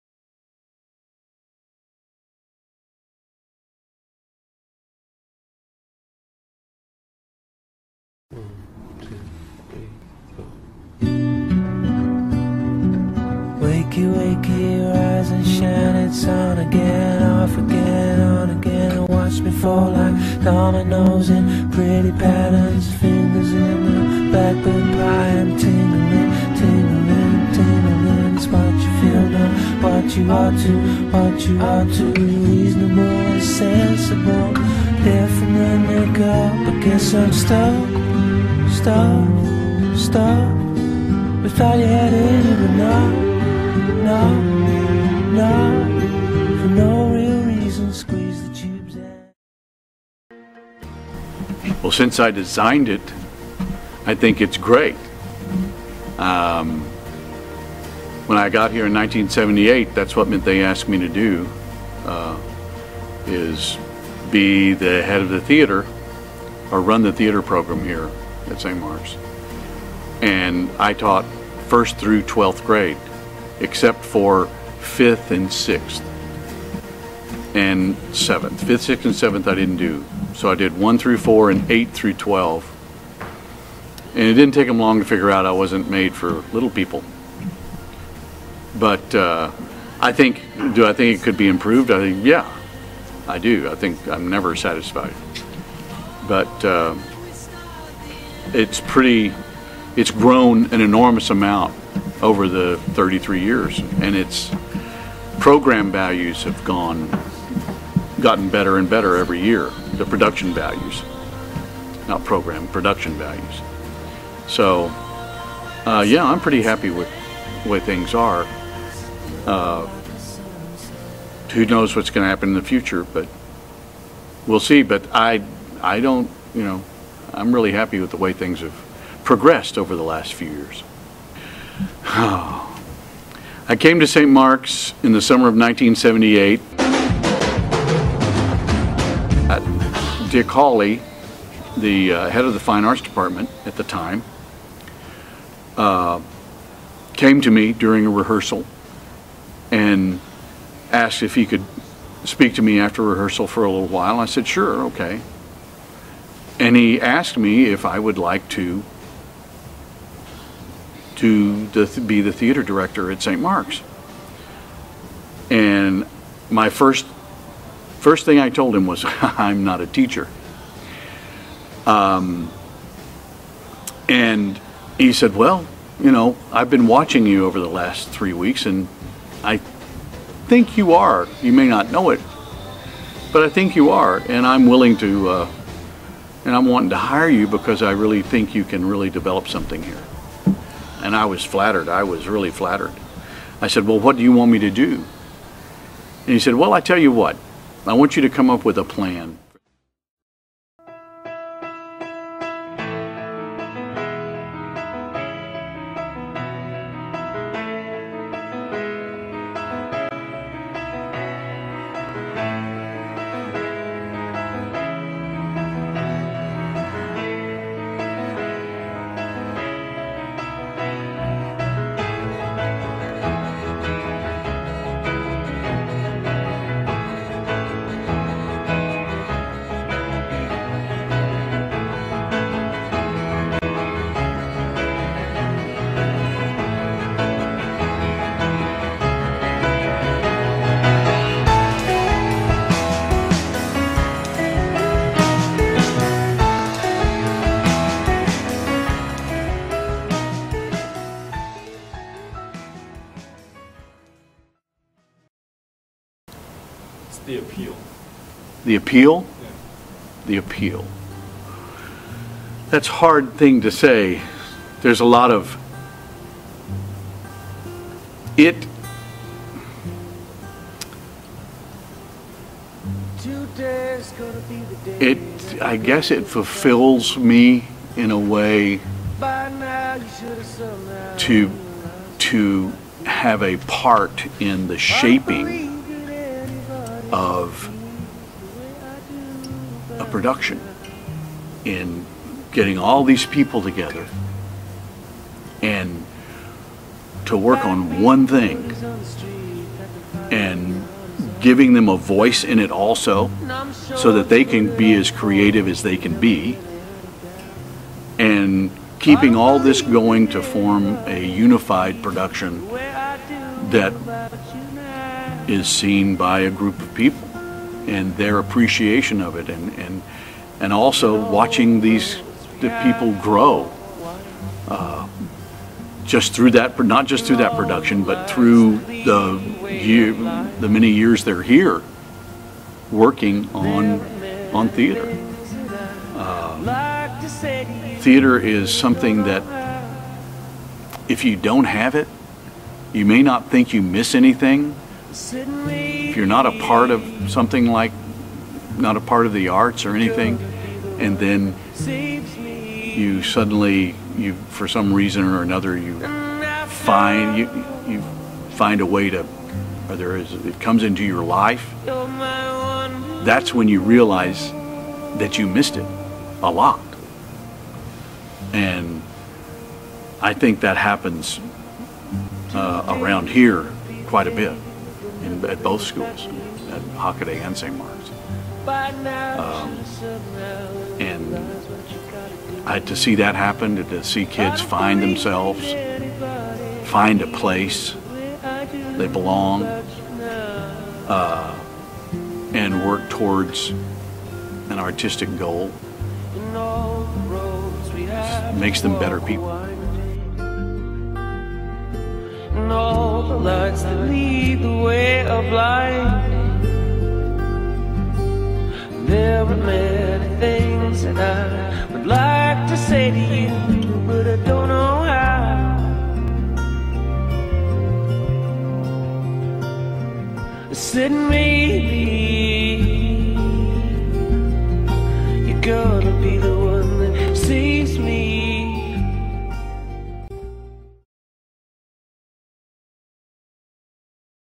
Blaydes
Rod Blaydes, the St. Mark's and Hockaday theater teacher and mentor.